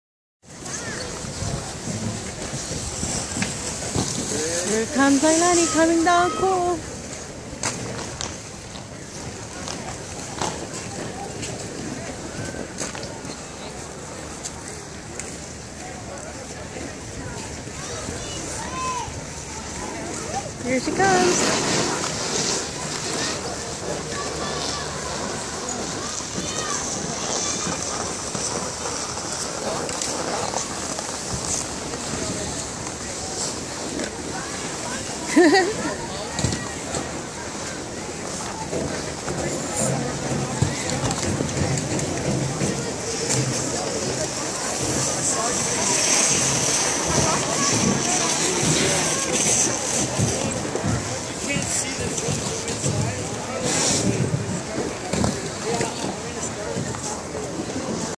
Buck Hill Ski resort #2
At Buck Hill Ski resort in Burnsville, Minnesota USA 12 /31/2014